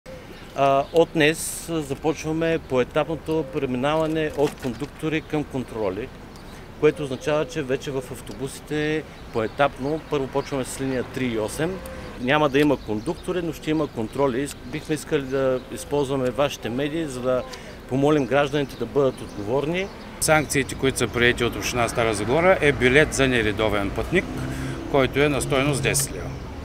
Контрольори заменят кондукторите в градския транспорт в Стара Загора
Кондукторите в градския транспорт в Стара Загора ще бъдат поетапно пренасочени на позиция контрольор. Това съобщи пред журналисти ресорният заместник-кмет Радостин Танев. Промяната започва с линии № 3 и 8. Радостин Танев призова ползващите услугите на „Тролейбусни и автобусни превози“ да бъдат отговорни и заяви, че самотаксуването е задължително. Глобата за нередовен пътник е 10 лева.

Закупуването на билет се извършва в билетните центрове, чрез мобилното приложение и от устройствата в тролейбусите и автобусите.

„Работим за увеличаване на броя на касите, в които хората могат да зареждат картите си. Предстои и поставяне на устройства в колите на градския транспорт, които ще дават възможност за плащане с карта“ – добави Радостин Танев.

„В момента общинското транспортно дружество разполага със 110 шофьори, 94-ма кондуктори и 12 контрольори. За удобство на пътниците в горещите дни осигуряваме безплатна минерална вода“ – сподели управителят на „Тролейбусни и автобусни превози“ инж. Георги Станев.